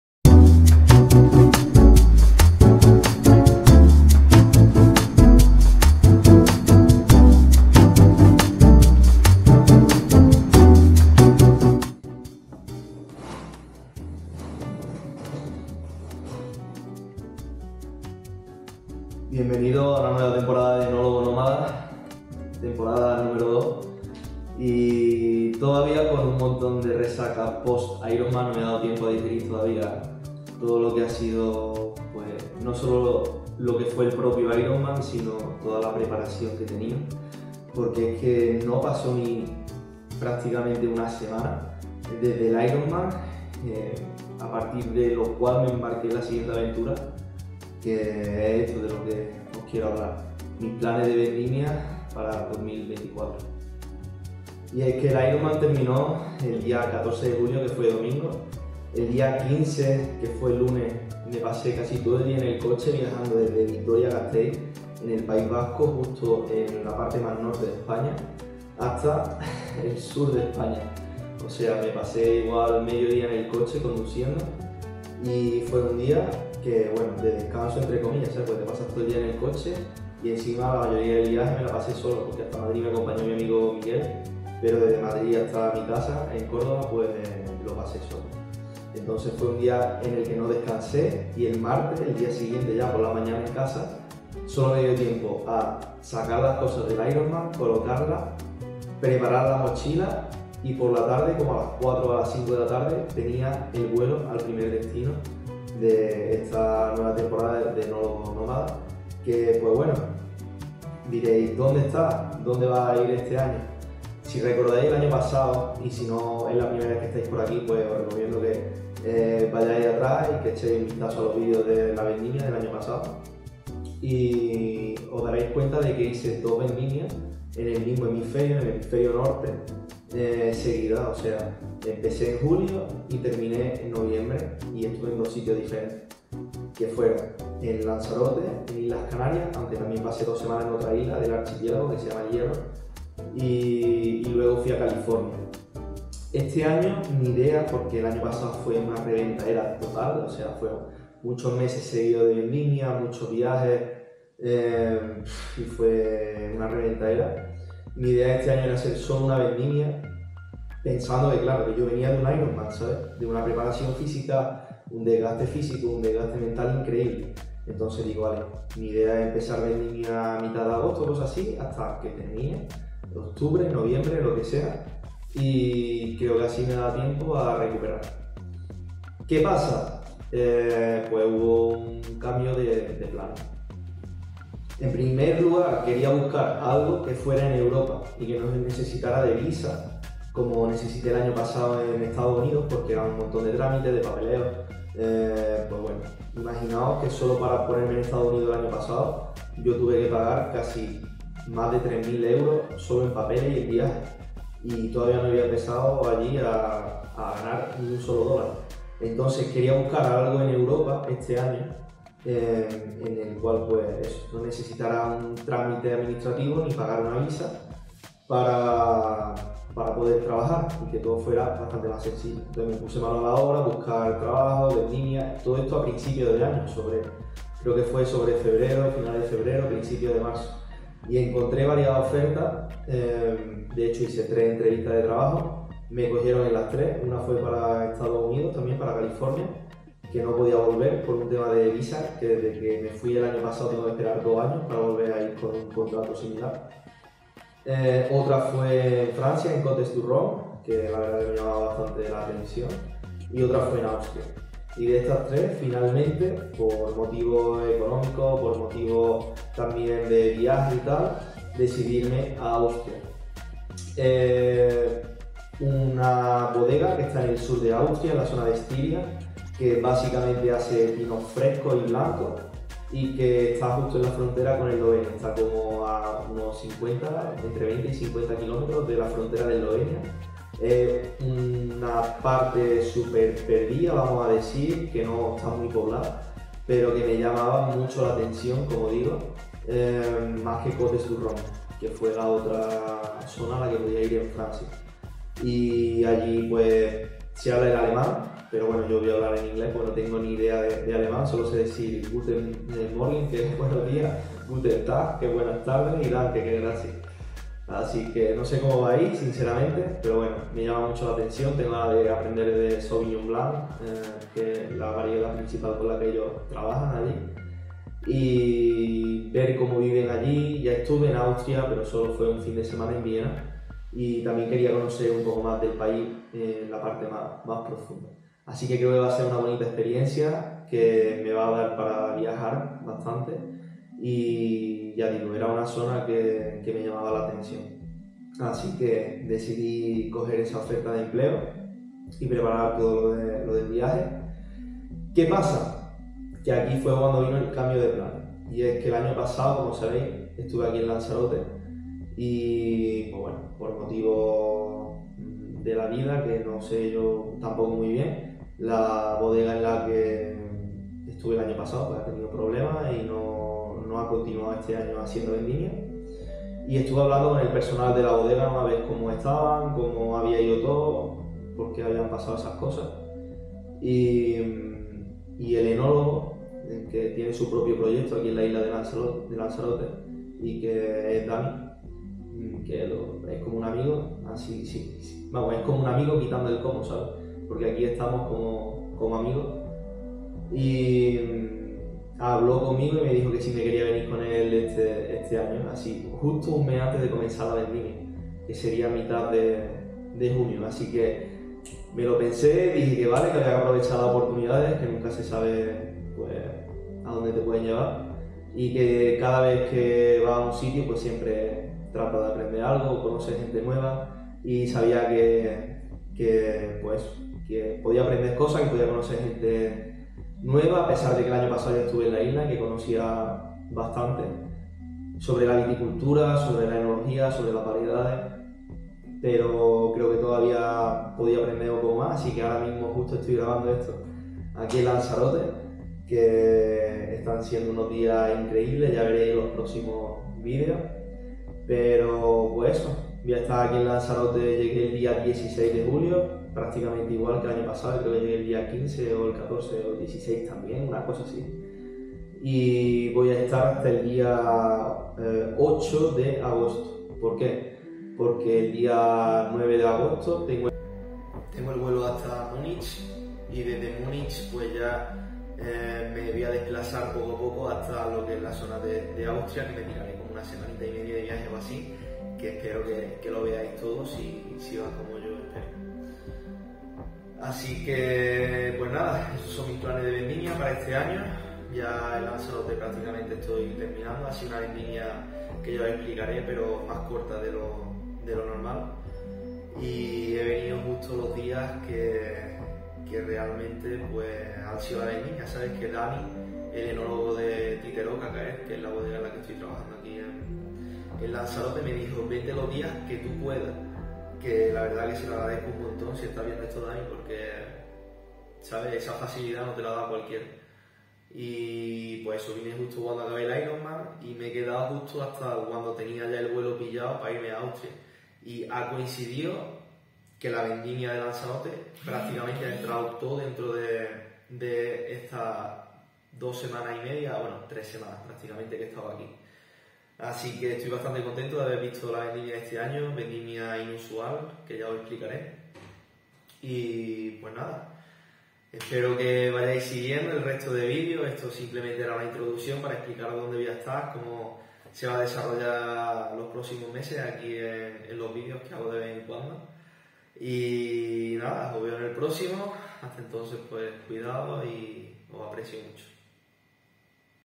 PLANES VENDIMIA 2024 | ENÓLOGO NÓMADA T.02
Explicación de mis planes para la vendimia de 2024.

Sigue en pie el plan de recorrer el mundo mientras hago vino. Lo tenía claro cuando empecé la temporada 01.